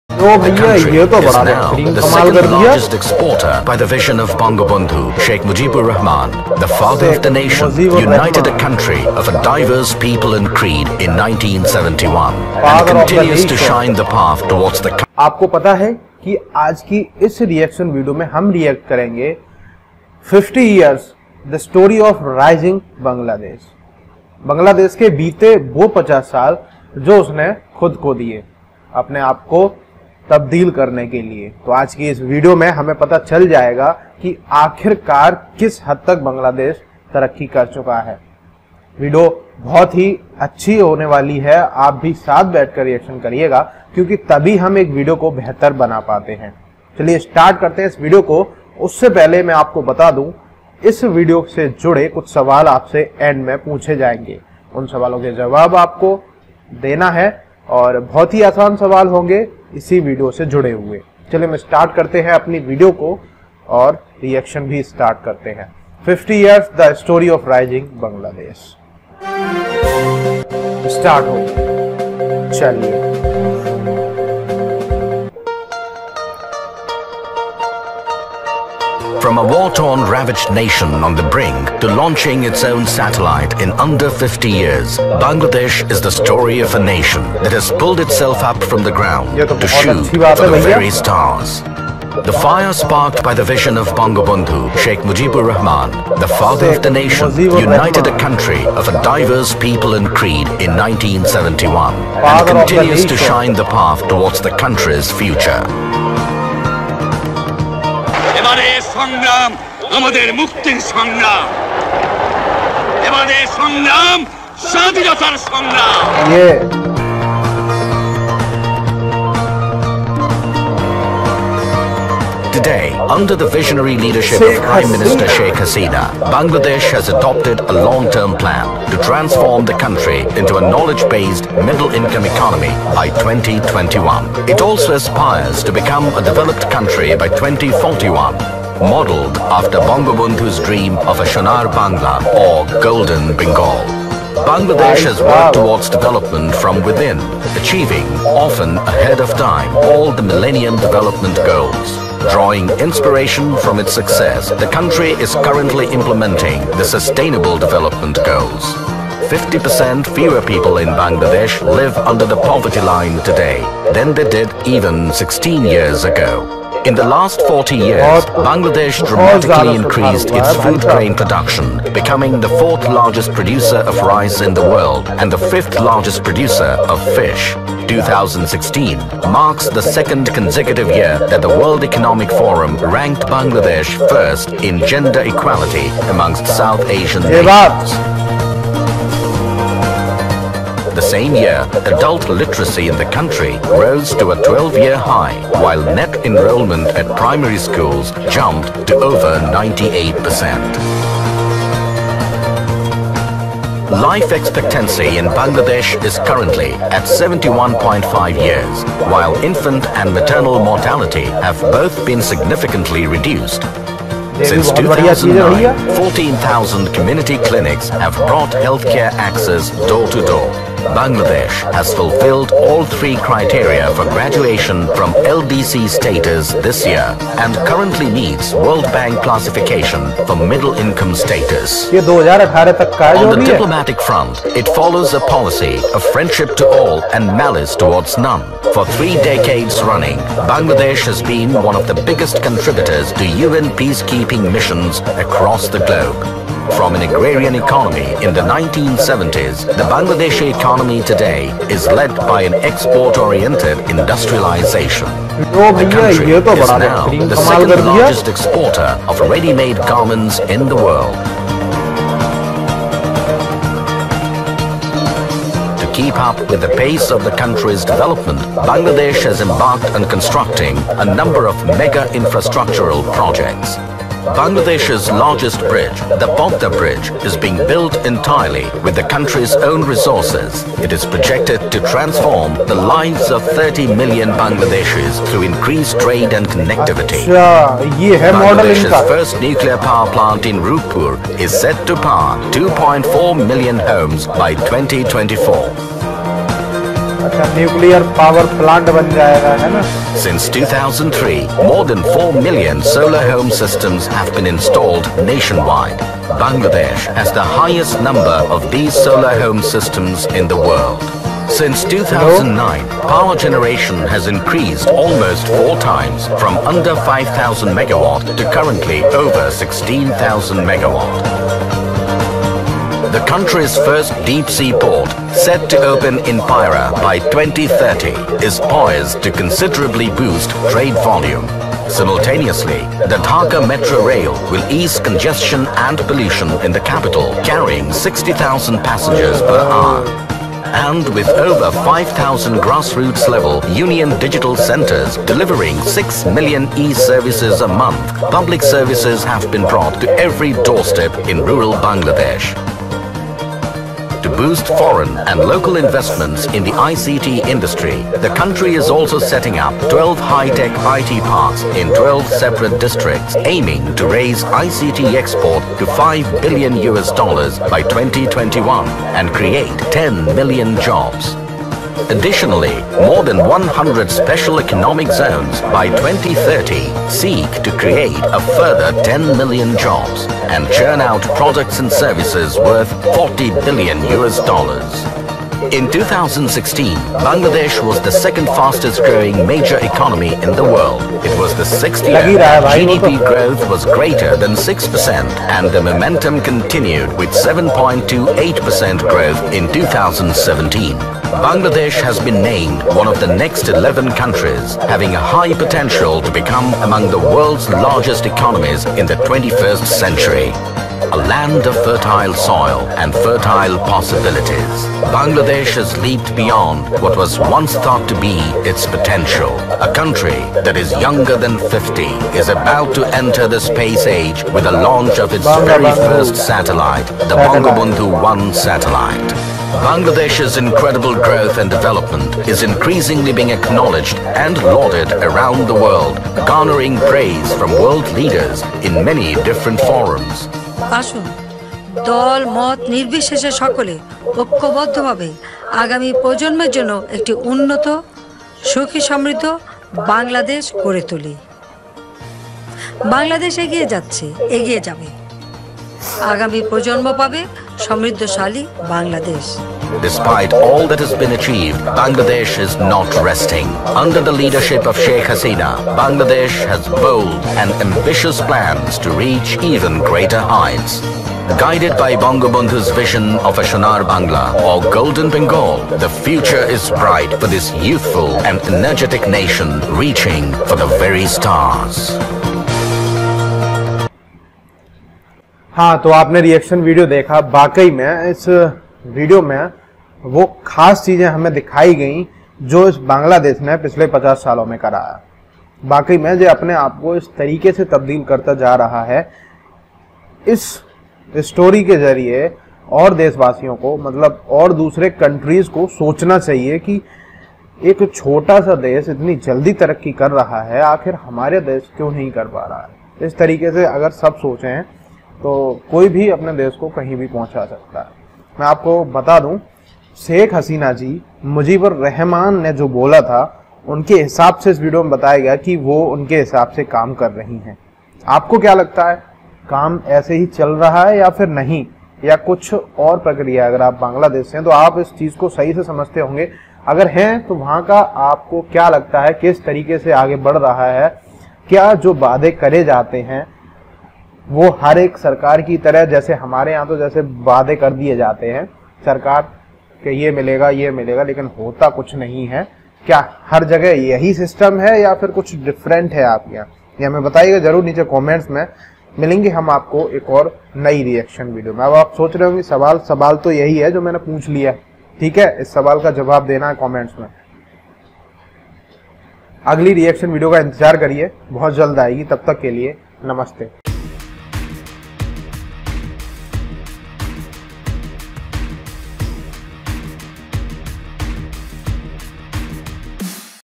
Indian Reaction on 50 years the story of rising Bangladesh
Today we are checking out 50 years the story of rising Bangladesh

opinion, views and reactions over different videos to entertain People.